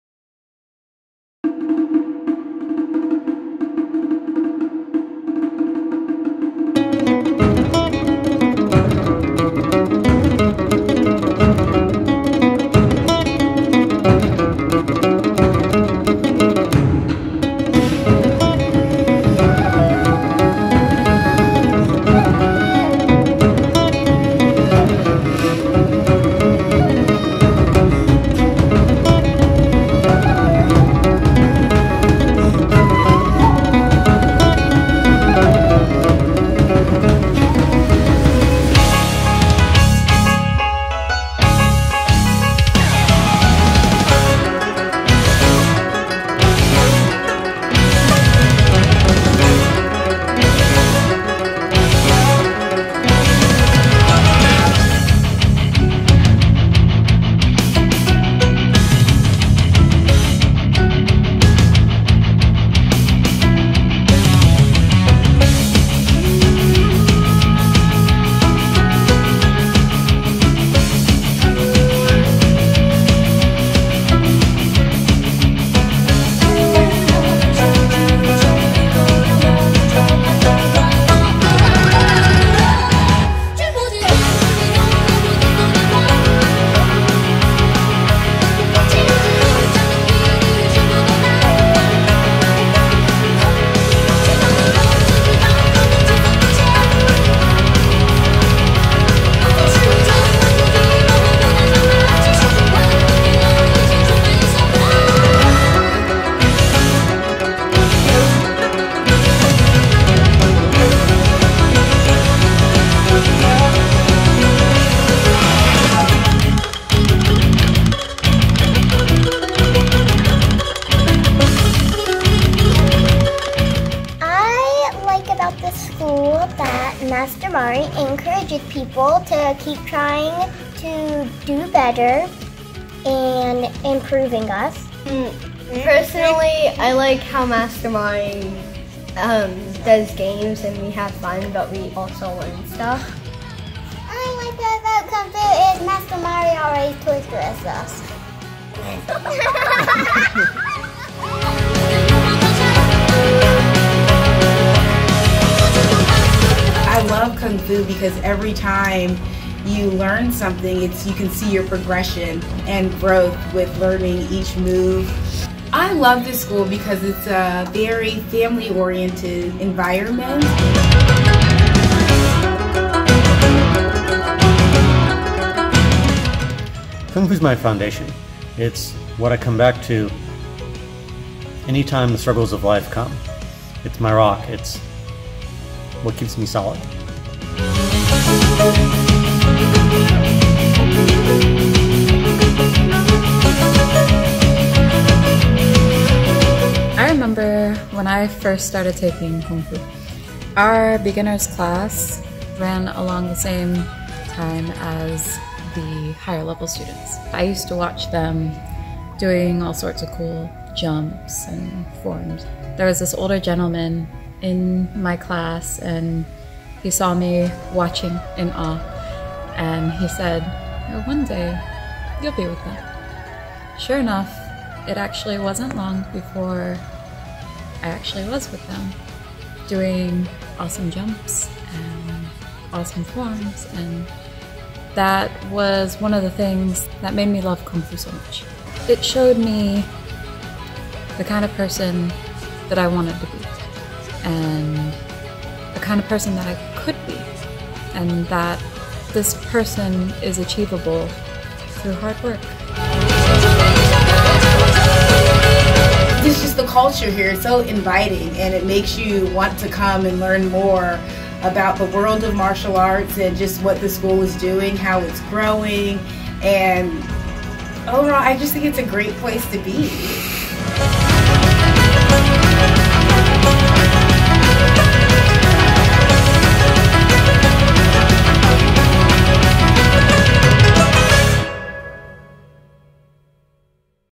Step into East Cloud
East Cloud Kungfu, House of Chinese martial arts in Raleigh, NC.